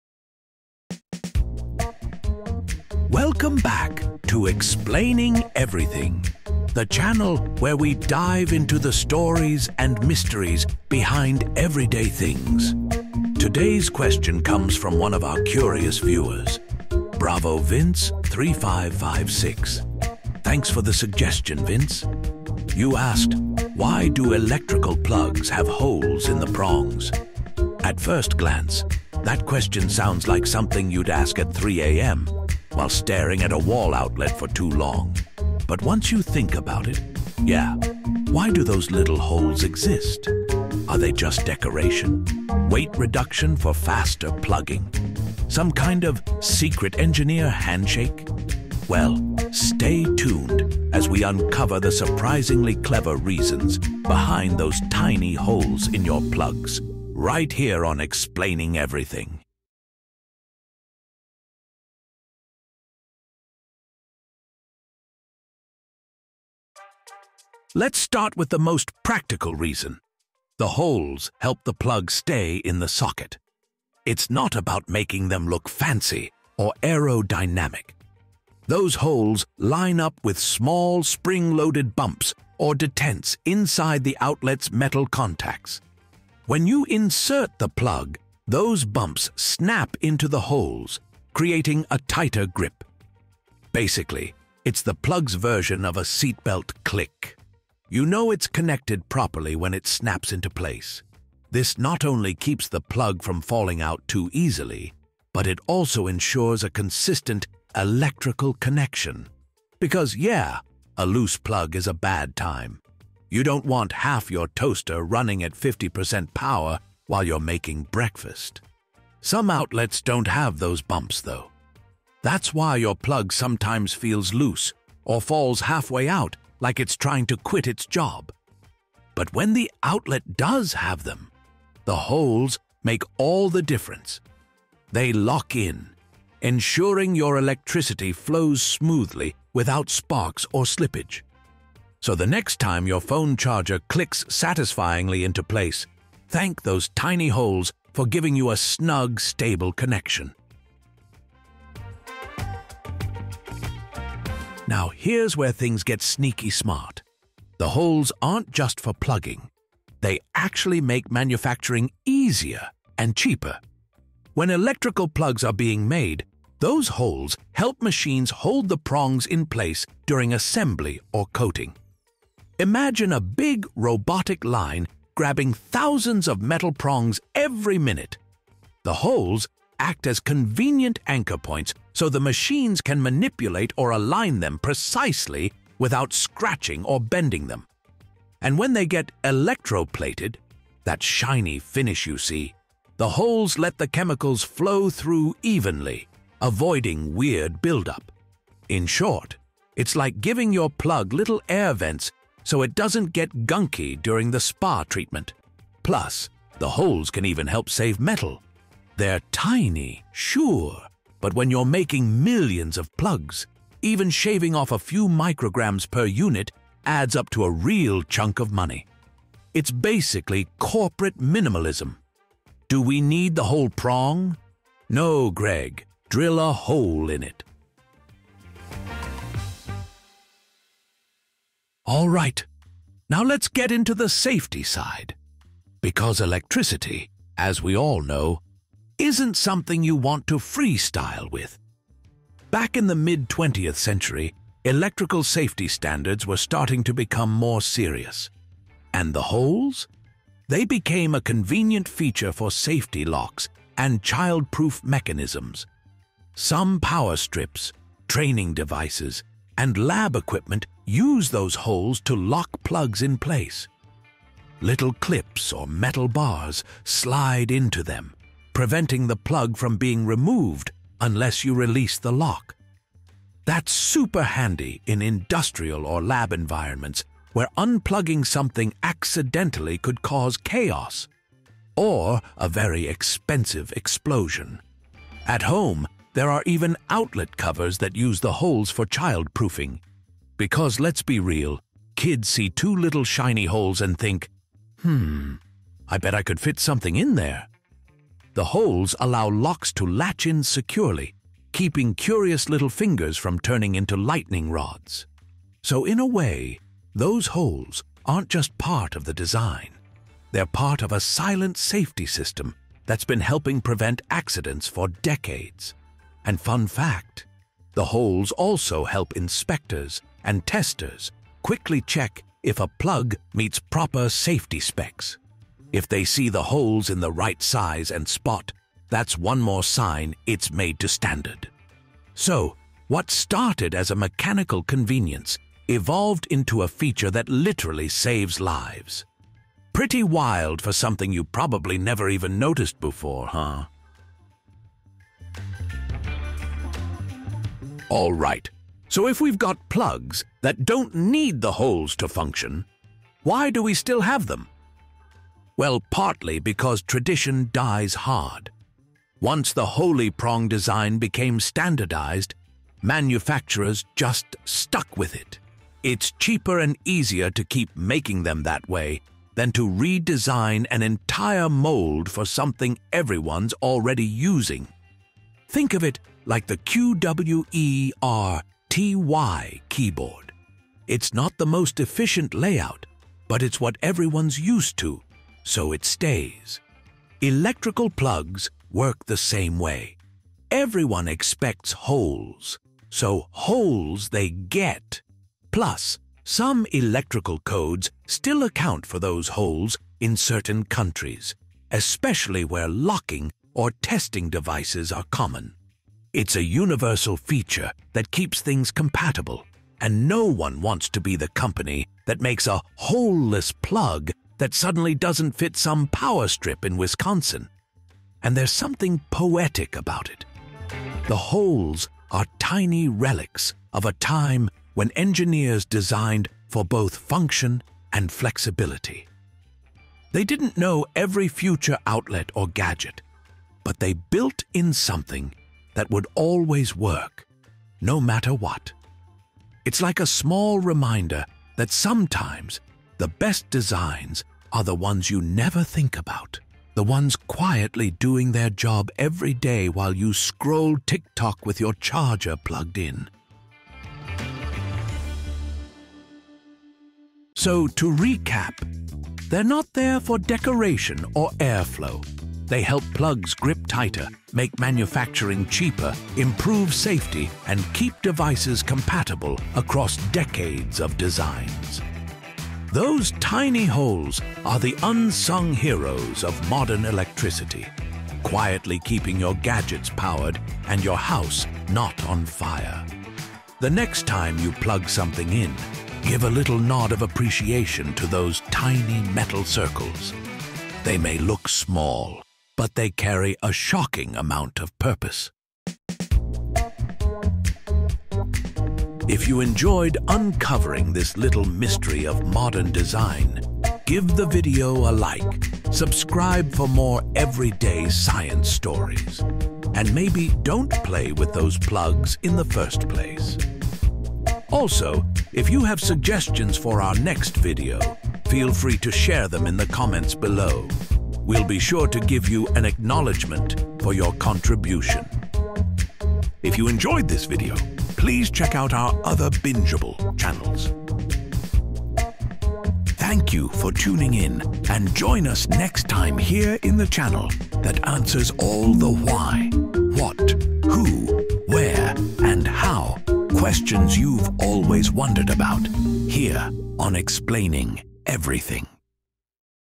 Why Do Electrical Plugs Have Holes in the Prongs? Tiny Holes, Shocking Purpose
Why do electrical plugs have tiny holes in their prongs? 🤔 You’ve probably seen them your whole life — on phone chargers, toasters, lamps — but never stopped to ask why they’re there. In today’s episode, we’re uncovering the shocking truth (pun absolutely intended) behind this small but genius design feature.

Timestamps:
0:03 – Intro
1:06 – The gripping truth
2:34 – Manufacturing magic and money-saving tricks
3:51 – Safety features and sneaky plug locks
5:47 – Why we still use holes today
7:37 – Conclusion

Video Edited By:
Matthew M.